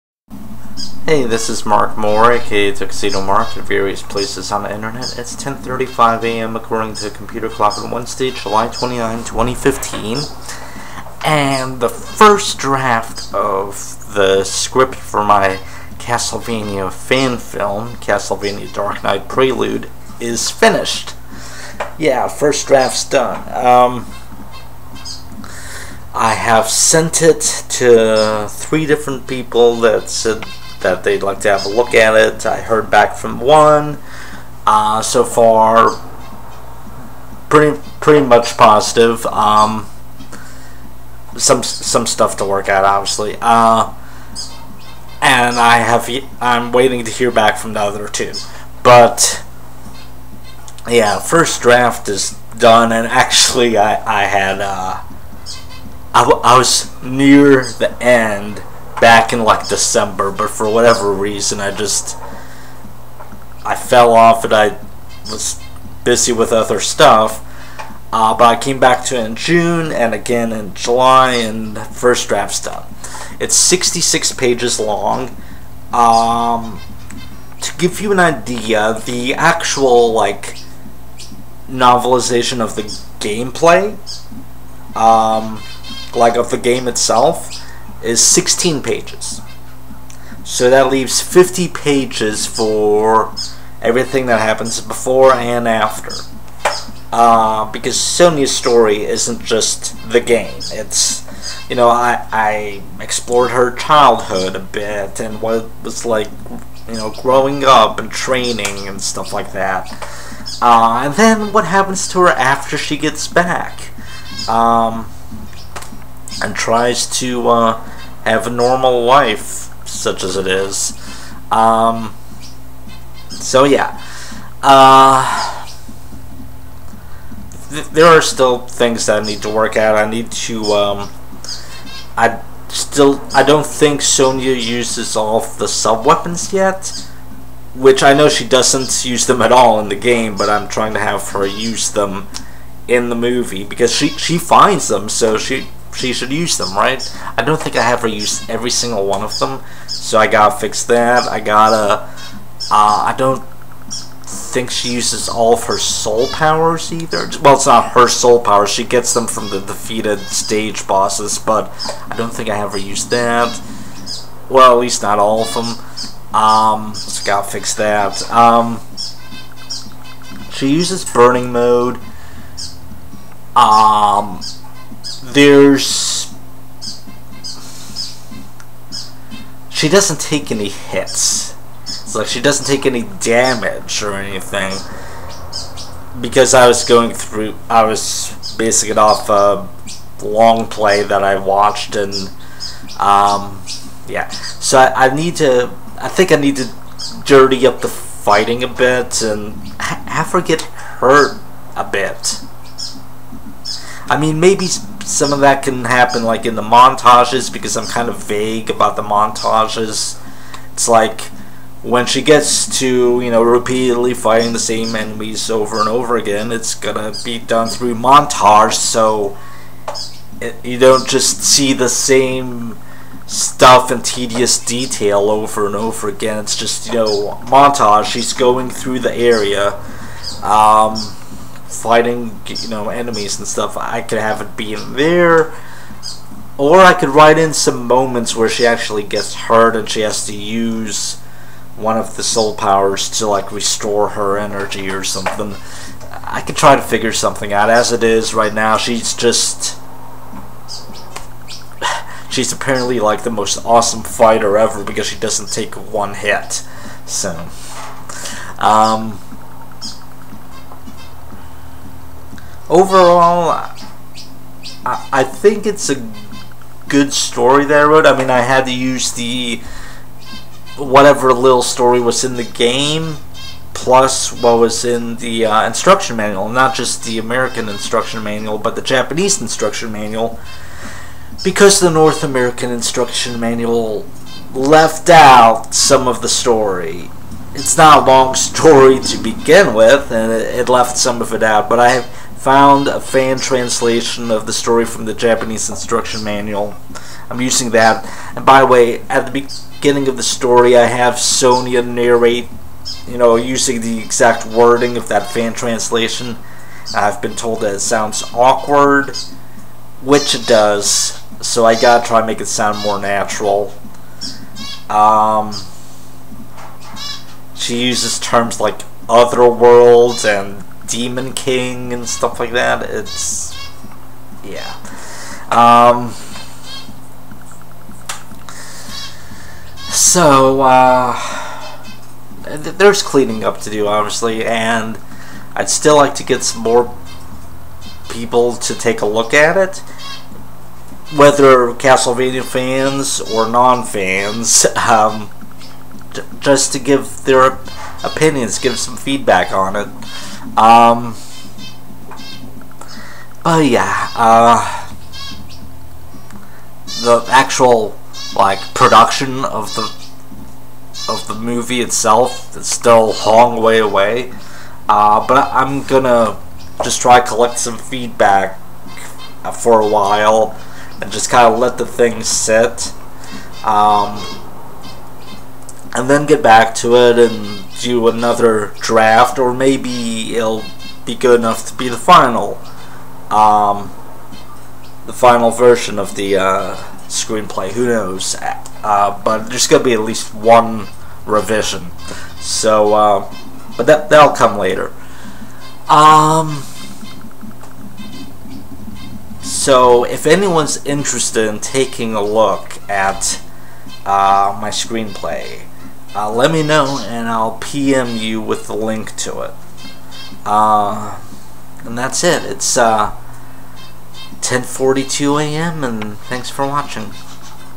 VLOG: Castlevania fan film - first draft complete!
I have finished the first draft of my screenplay for my fan film, "Castlevania: Dark Night Prelude". I'm looking for fans and non-fans to read it and give feedback.